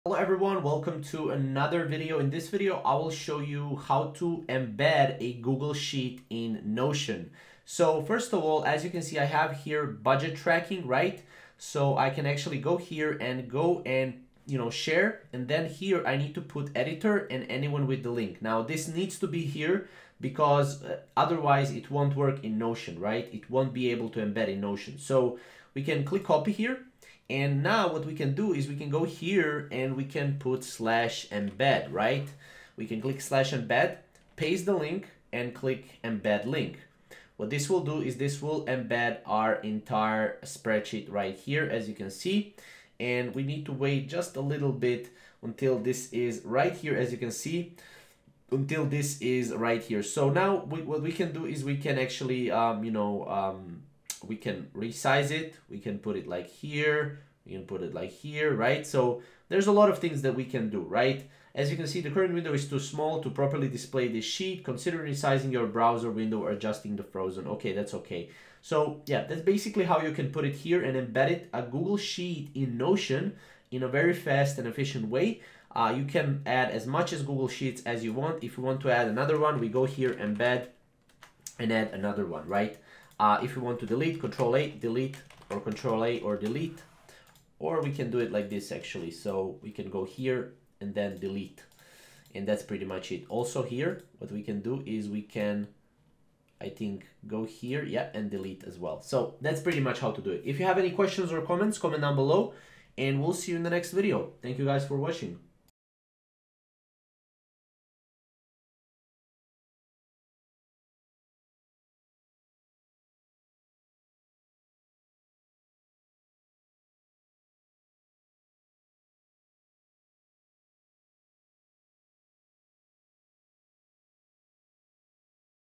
How to Embed a Google Sheet in Notion (2024)
In this video, I will show you how to embed a google sheet in notion